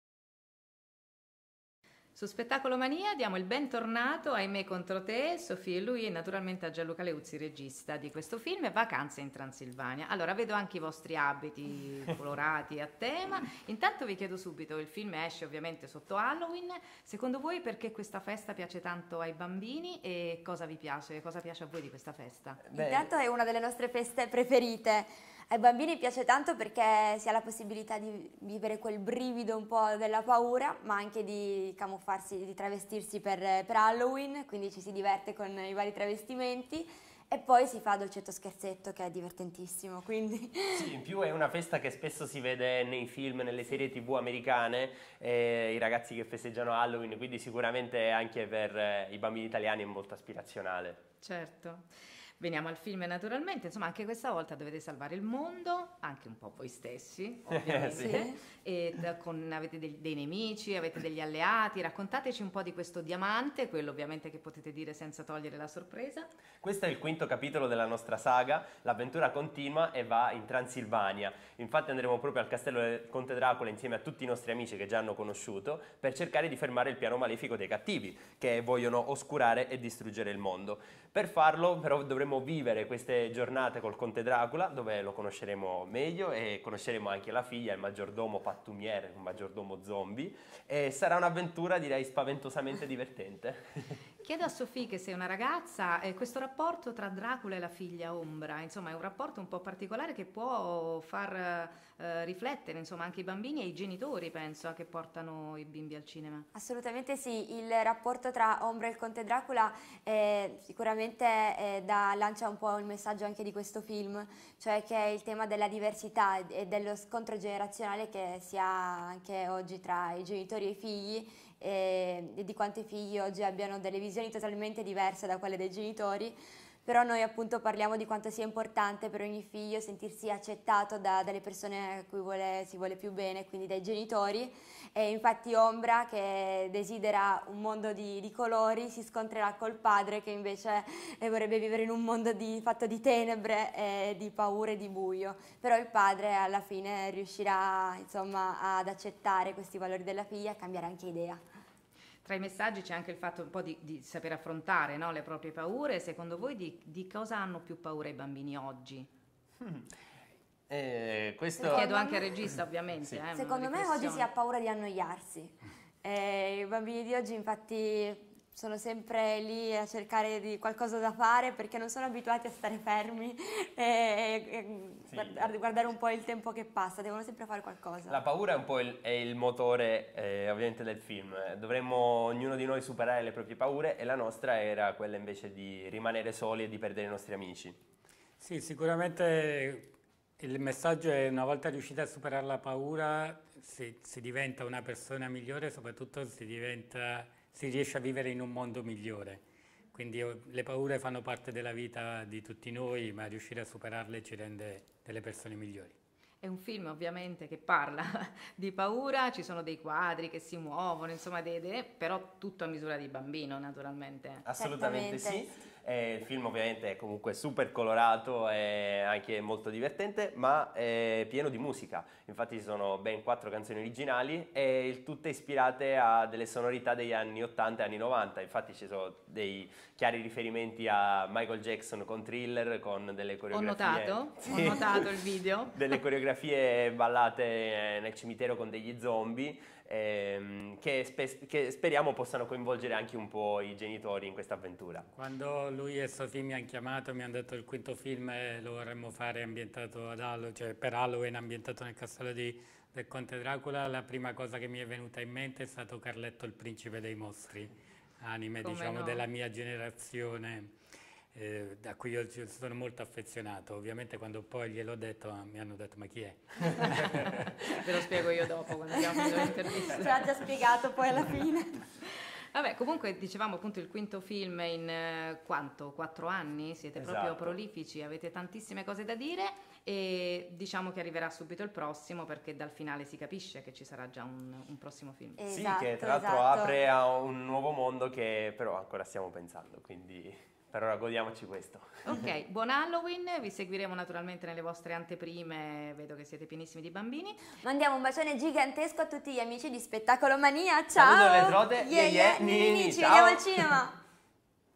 Me contro Te Vacanze in Transilvania: videointervista a Sofì, Luì e Gianluca Leuzzi
Videointervista ai Me contro Te e Gianluca Leuzzi per 'Vacanze in Transilvania'